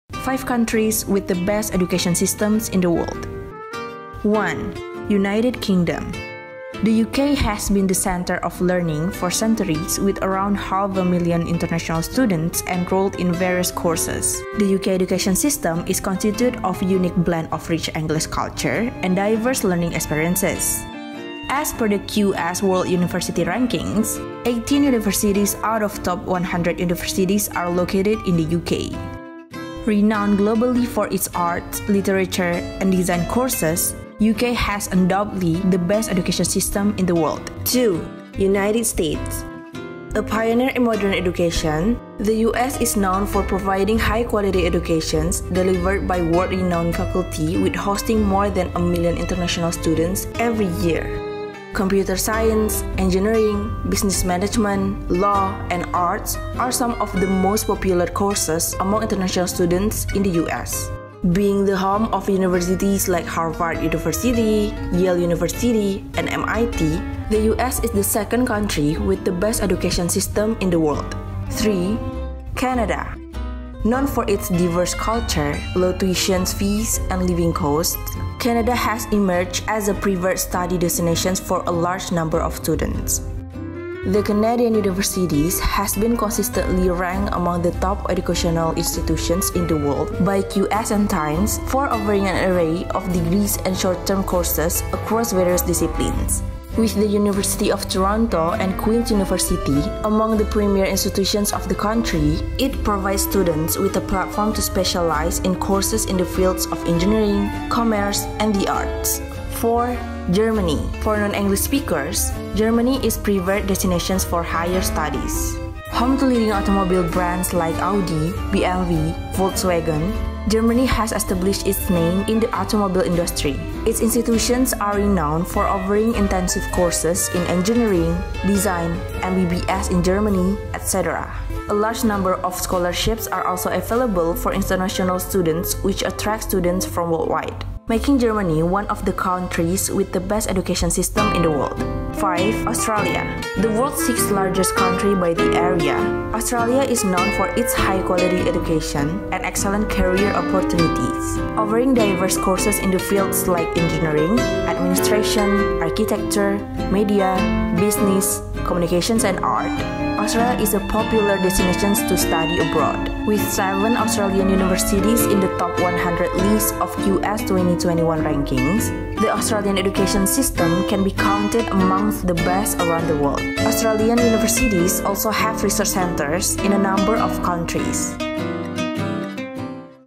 5 Countries with the Best Education Systems in the World
When it comes to pursuing higher education from abroad, every country has its own set of pros and cons. While one country may score well on the infrastructure, the other may offer degree programs that are new and unique. Thus, it can be slightly difficult to decide which country offers high-quality education. Based on the parameters like the types of programs offered, interdisciplinarity in programs, placements, university rankings, and the number of enrolled international students, we have curated a list of countries that are known for having the best education system.